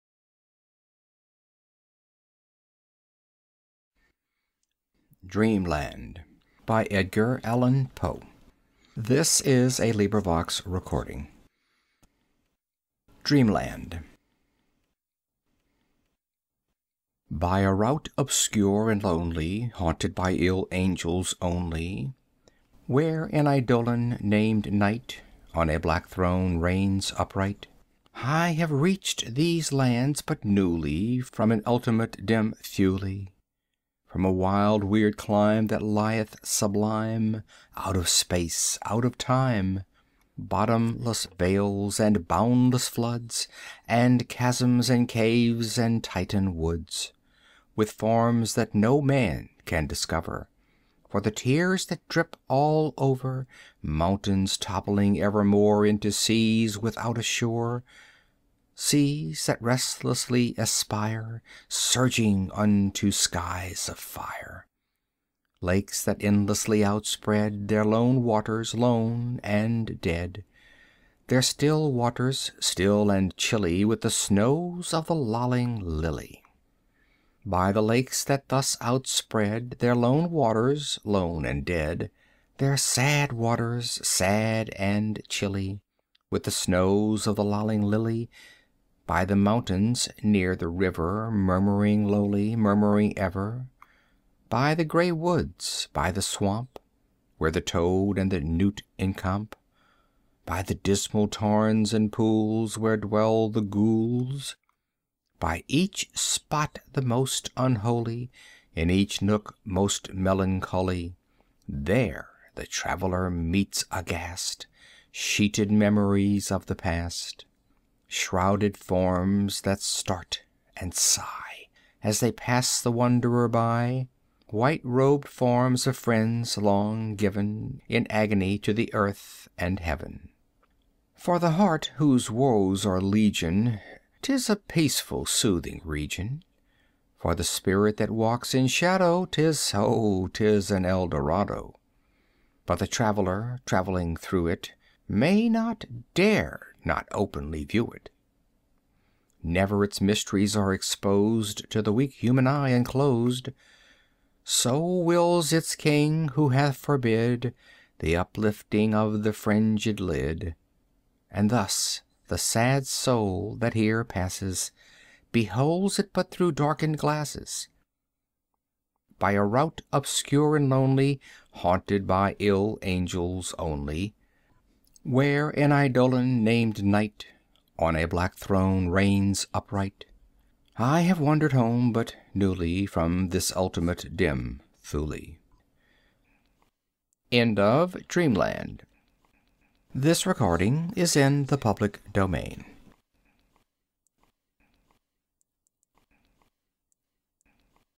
The Raven and Other Poems by Edgar Allan Poe - 7/12. Dreamland (read by Phil Chenevert)
LibriVox recording of The Raven and Other Poems, by Edgar Allan Poe. Read by Phil Chenevert.